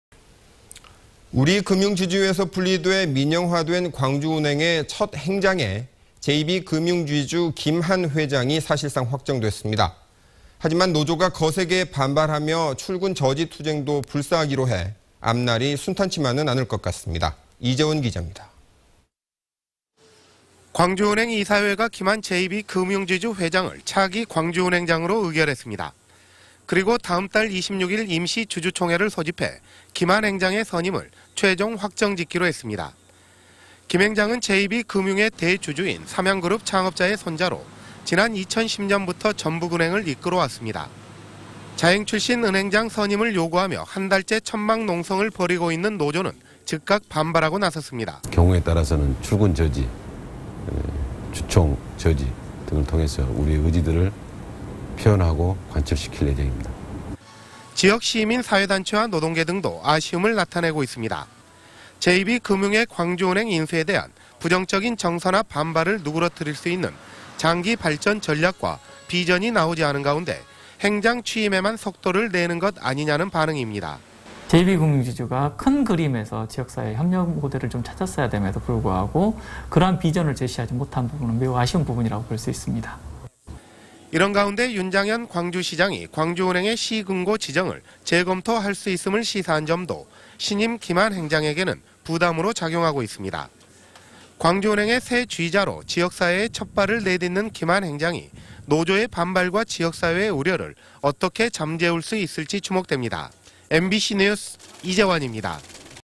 [뉴스데스크]광주은행 새 행장에 김한 JB 회장
◀ＡＮＣ▶
우리 금융지주에서 분리돼 민영화된
광주은행의 첫 행장에
김한 JB 금융지주 회장이 사실상 확정됐습니다.

하지만 노조가 거세게 반발하며
출근 저지 투쟁도 불사하기로 해
앞날이 순탄하지만은 않을 것 같습니다.

이재원 기잡니다.

           ◀ＥＮＤ▶
           ◀ＶＣＲ▶

광주은행 이사회가 김한 JB 금융지주 회장을
차기 광주은행장으로 의결했습니다.

그리고 다음 달 26일 임시 주주총회를 소집해
김 한 행장의 선임을 최종 확정짓기로 했습니다.

김 행장은 JB 금융의 대주주인
삼양 그룹 창업자의 손자로
지난 2010년부터 전북 은행을 이끌어 왔습니다.

자행 출신 은행장 선임을 요구하며
한 달째 천막 농성을 벌이고 있는 노조는
즉각 반발하고 나섰습니다.

           ◀ＩＮＴ▶

지역 시민사회단체와 노동계등도
아쉬움을 나타내고 있습니다.

JB 금융의 광주은행 인수에 대한
부정적 정서나 반발을 누그러뜨릴 수 있는
장기 발전 전략과 비젼은 내놓지 않은 가운데
행장 취임에만 속도를 내는것 아니냐는 반응입니다.

           ◀ＩＮＴ▶

이런 가운데 윤장현 광주시장이
광주 은행의 시금고 지정을
재검토 할 수 있음을 시사한 점도
신임 김 한 행장에게는 부담으로
작용하고 있습니다.

광주 은행의 새 지휘자로
지역 사회에 첫발을 내딛는 김한 행장이
노조의 반발과 지역 사회의 우려를
어떻게 잠재울 수 있을지 주목됩니다.

MBC뉴스